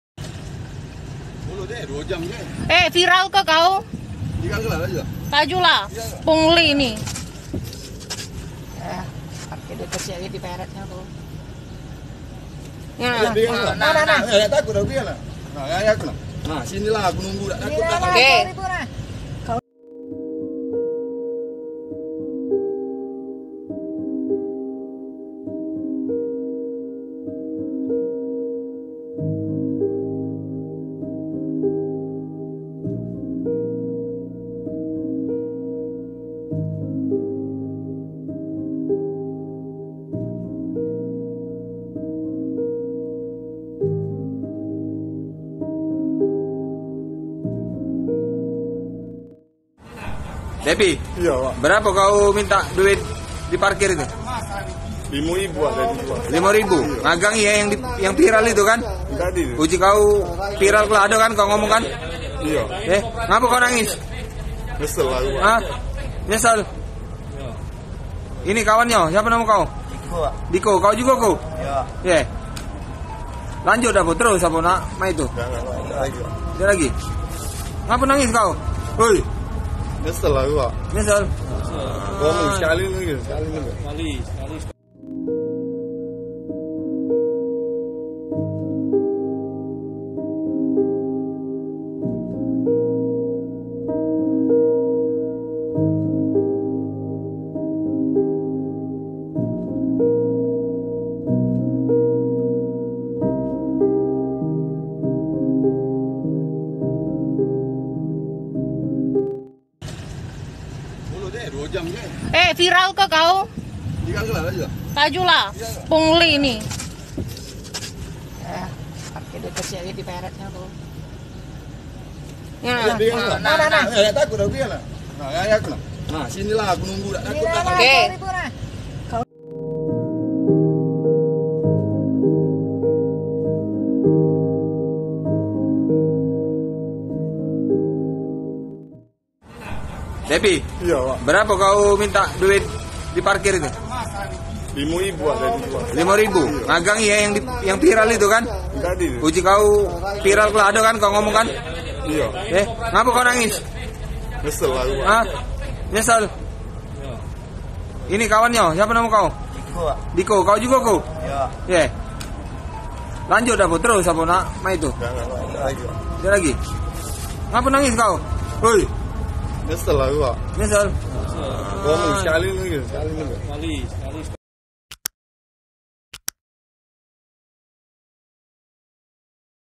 Jukir Peras Korban Minta Rp 20 Ribu, Menangis saat Diamankan Warga, Nyesel Aku!
TRIBUN-TIMUR.COM - Viral video juru parkir atau jukir diamankan oleh warga di Palembang, Sumatera Selatan.

Rekaman itu diunggah akun Instagram @palembangkulukilir pada Selasa, 4 Januari 2022.

Terlihat dua orang pria bertato telah diamankan.

Dalam keterangan unggahan disebutkan dua jukir tersebut telah melakukan pemerasan terhadap sejumlah warga.

Ia memeras uang parkir Rp 10 ribu hingga Rp 20 ribu yang dinilai warga berlebihan.

Setelah diamankan, salah satu di antara jukir tersebut nampak menangis.

Ditanya alasannya menangis, sang jukir mengaku menyesal telah melakukan pemerasan.

Bahkan ia mengaku aksi ini baru pertama kalinya dilakukan
Warganet berharap aksi pemerasaan ini tidak terjadi lagi. (*)

Video Editor: Octa Chandra J